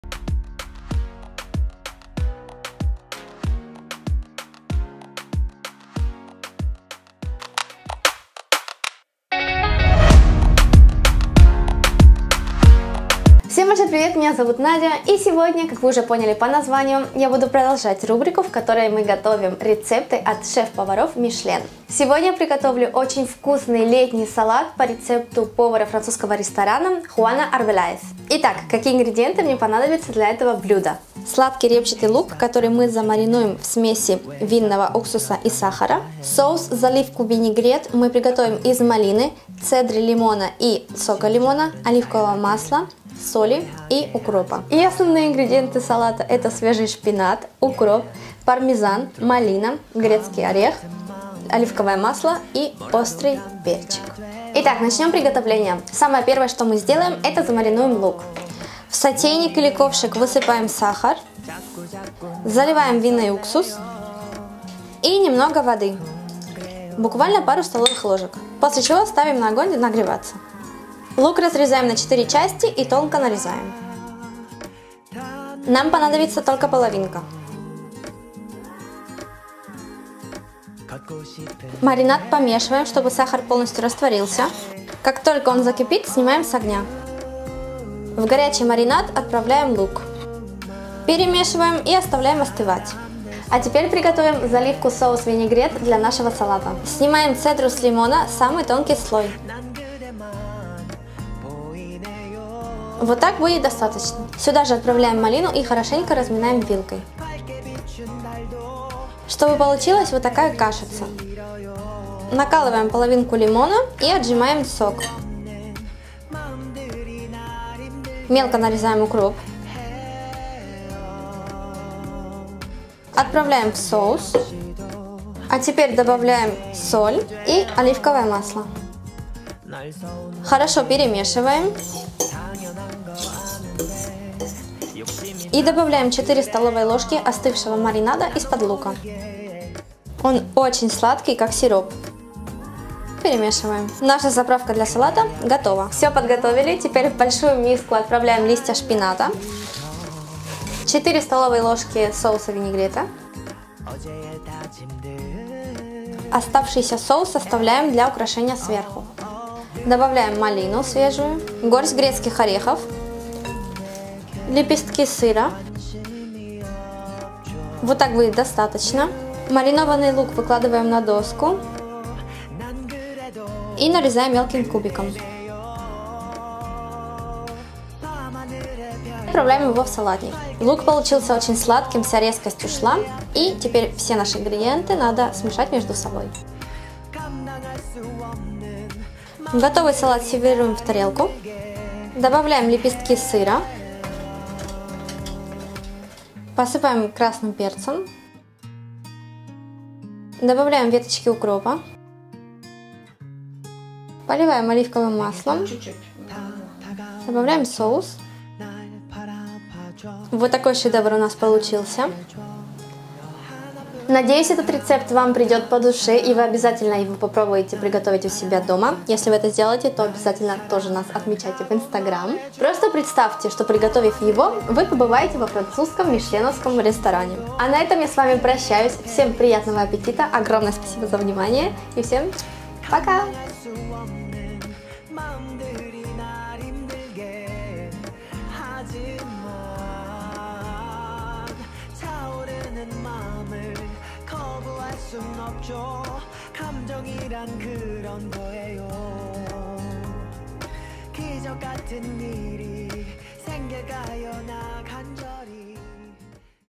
Легкий Салат "Парижанка". Мишлен дома. Простой рецепт.
Как приготовить вкуснейший летний салат по рецепту шеф повара Мишлен Juan Arbeláez.
Легкий полезный и очень вкусный салат из шпината, сыра, грецких орехов и малины. Побалуйте себя роскошным салатом из доступных продуктов.

Ингредиенты для 2 порций:
125 гр молодые листья шпината
50 гр грецкие орехи
15 шт ягоды малины
30 гр сыр пармезан
1/2 шт лук сладкий
маринад для лука:
20 мл винный уксус
50 гр сахар
10 мл вода
для соуса vinаigrette:
1 шт лимон (цедра и сок 1/2 шт)
15 шт малина
2 ст.л. оливковое масло
щепотка соли
2 веточки укропа
4 ст.л. готового остывшего маринада
украшение:
10 гр сыр пармезан
щепотка красного перца
1 ст.л. оливкового масла
4 веточки укропа